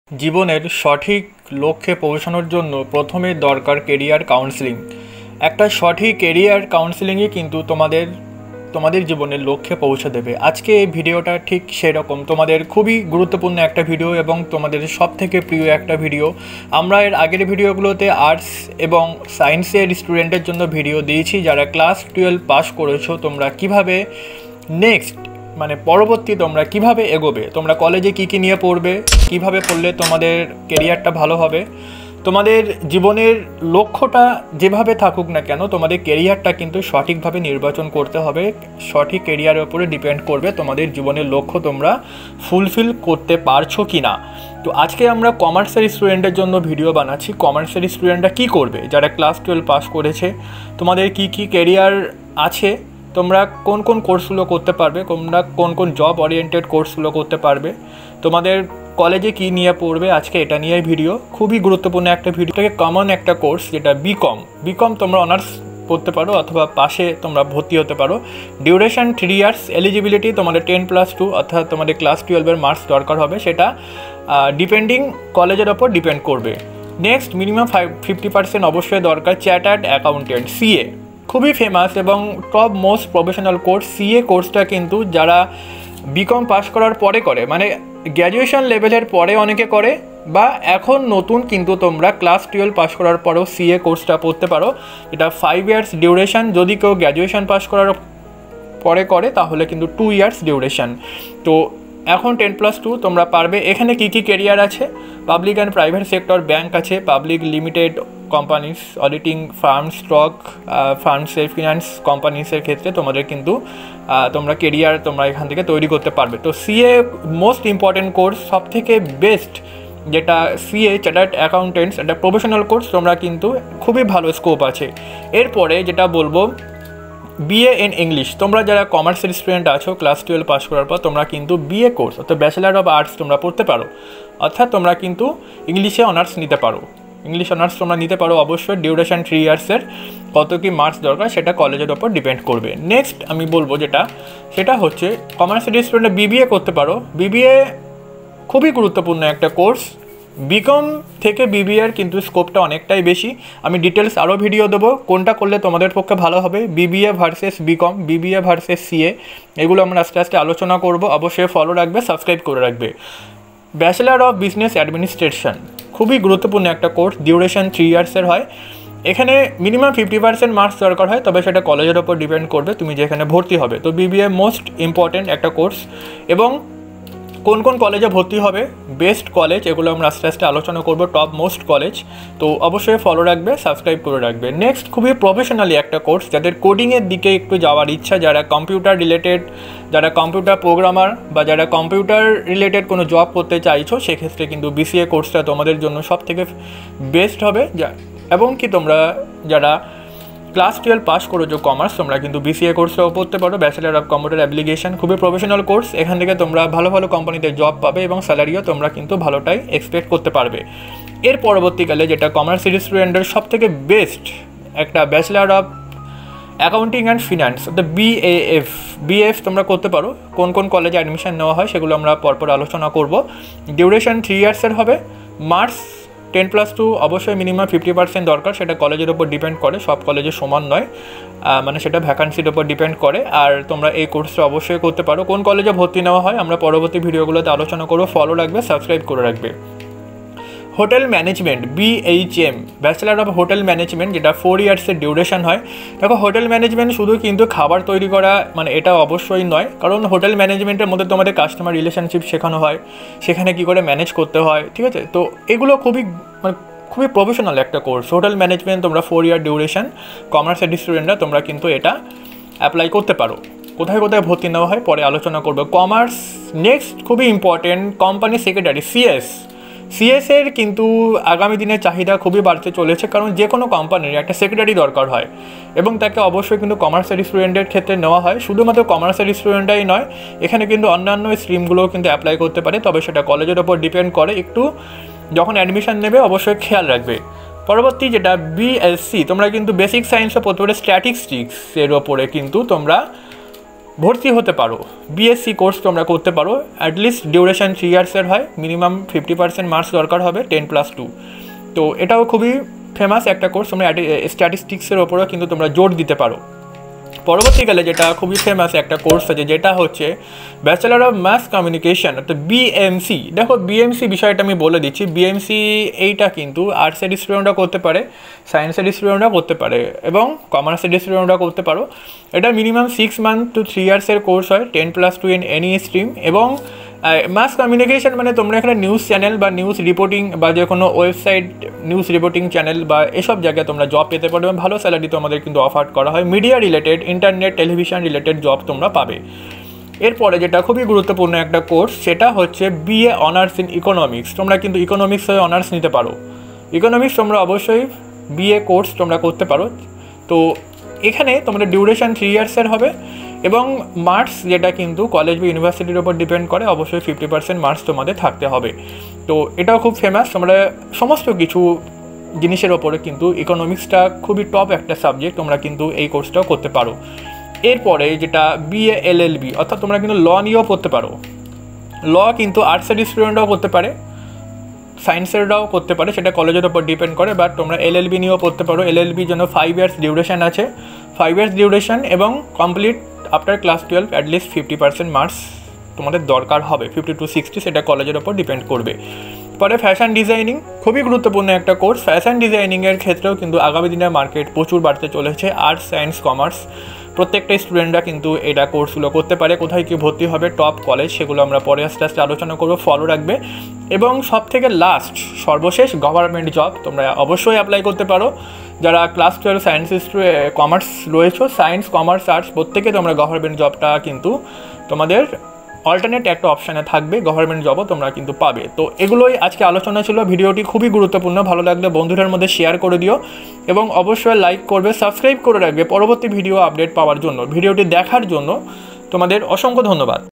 What to do after 12th Commerce Student | Best Career and Professional Courses in Bengali | 100% Job
Topic:- how to study, how to study smart, student motivational video for success